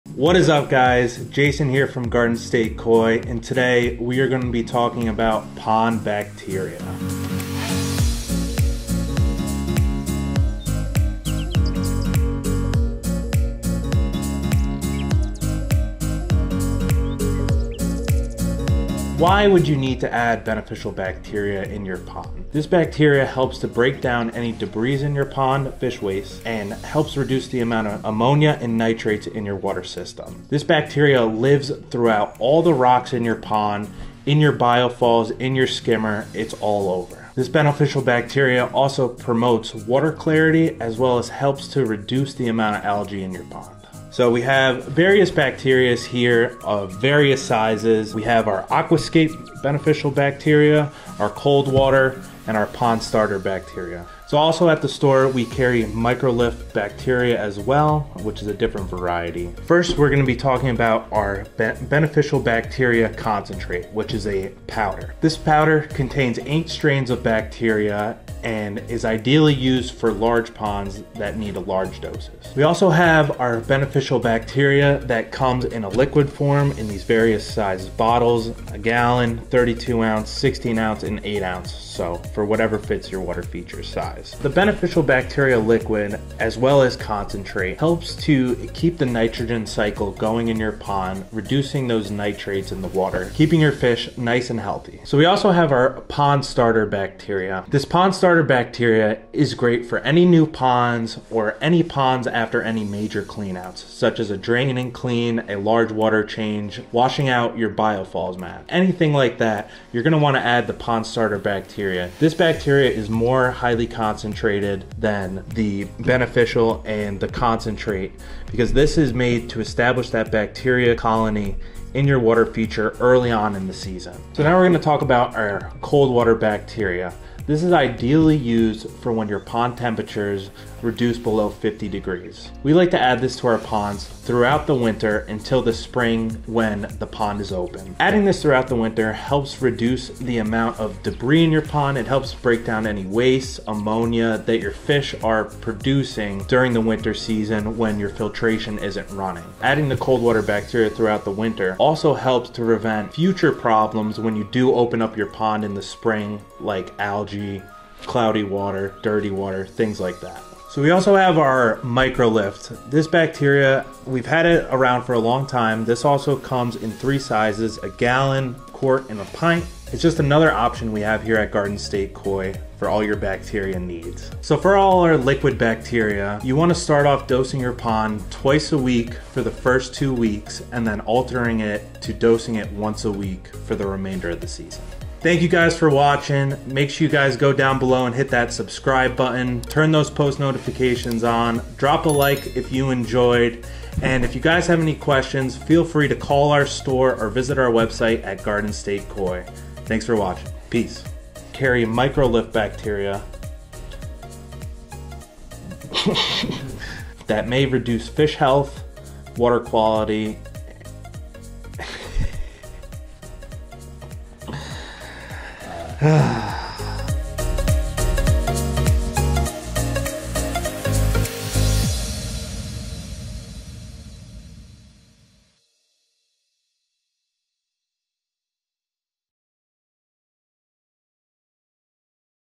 Guide to Beneficial Bacteria
Looking to learn about the benefits of beneficial bacteria for your koi pond/water garden? Look no further! Jason from Garden State Koi is here to teach you about all the different types of beneficial bacteria, what they do, and how you can use them to keep your pond's ecosystem thriving. In this video, Jason will go over Aquascape Cold Water Beneficial Bacteria, Aquascape Beneficial Bacteria Concentrate, and Aquascape Pond Starter Bacteria, as well as Microbe-Lift brand bacteria.